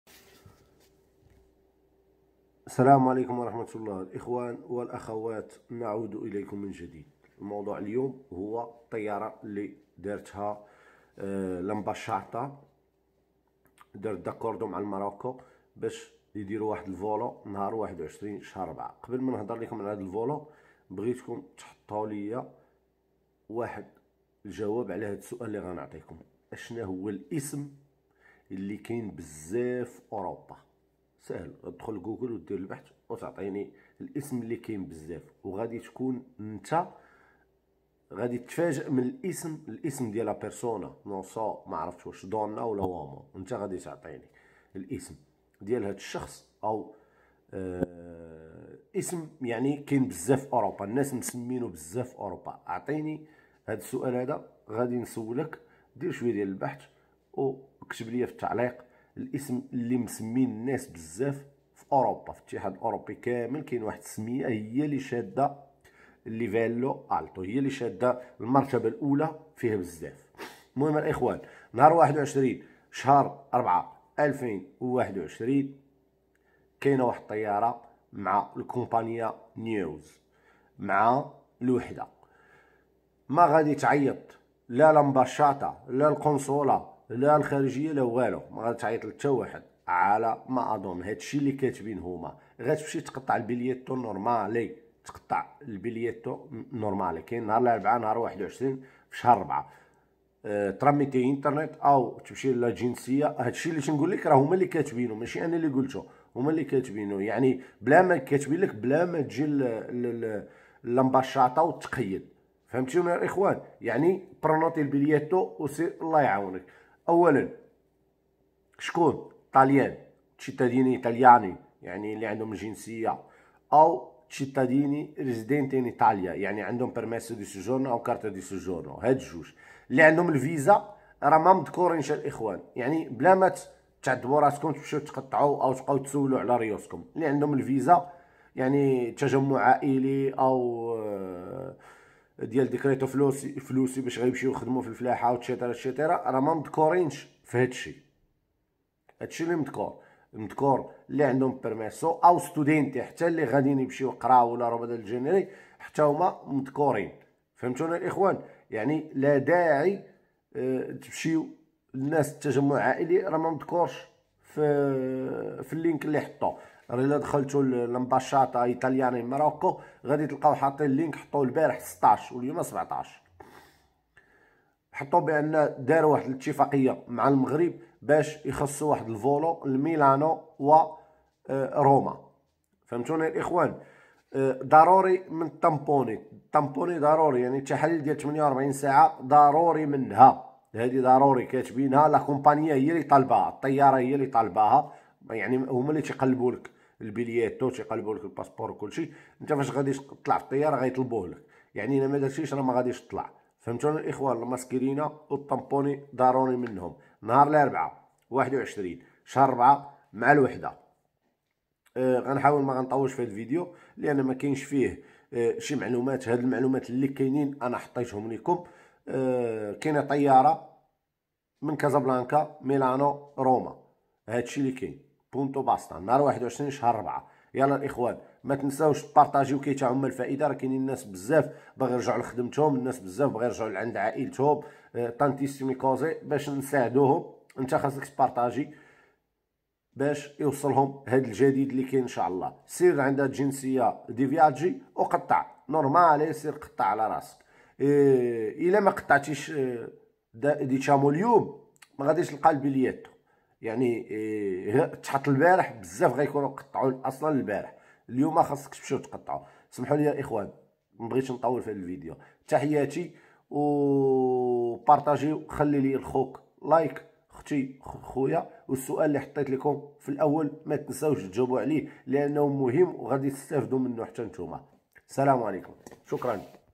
مواعيد رحلات الطيران من المغرب إلى إيطاليا
مواعيد رحلات الطيران

تعرف على مواعيد رحلات الطيران من المغرب الى ايطاليا
⛔✈️🛂🇮🇹 0

COVID19 | 🔴. VOLO COMMERCIALE SPECIALE DI RIENTRO IN ITALIA (21 APRILE 2021).

Al fine di rispondere all’esigenza dei cittadini italiani e dei cittadini marocchini residenti in Italia, in particolare di quanti hanno visto cancellati i propri voli di ritorno in Italia, l’Ambasciata ed il Consolato Generale a Casablanca, d’intesa con le Autorità marocchine, hanno chiesto alle compagnie aeree di valutare la possibilità di effettuare voli speciali commerciali diretti tra il Marocco e l’Italia.

Al riguardo si comunica che la compagnia Neos ha predisposto un volo commerciale speciale, mercoledì 21 aprile, sulla tratta Casablanca-Roma Fiumicino-Milano Malpensa.

I biglietti, prioritariamente riservati ai cittadini italiani e ai cittadini marocchini residenti in Italia, saranno acquistabili liberamente e direttamente (senza l’intervento dell’Ambasciata e del Consolato) sul sito internet della compagnia aerea Neos Air.

Ai cittadini marocchini residenti in Italia, si ricorda che è stata disposta un'ulteriore proroga fino al 30 aprile 2021 della validità dei permessi di soggiorno in scadenza/scaduti a partire dal 31 gennaio 2020. Solo per i titolari di tali permessi di soggiorno non sarà necessario munirsi di visto di reingresso.

I passeggeri a bordo devono indossare le mascherine chirurgiche.

A tutti coloro che rientrano in Italia si ricordano gli obblighi di autoisolamento. Per maggiori informazioni sulle misure in Italia e scaricare le autodichiarazioni, si invita a consultare il focus della Farnesina.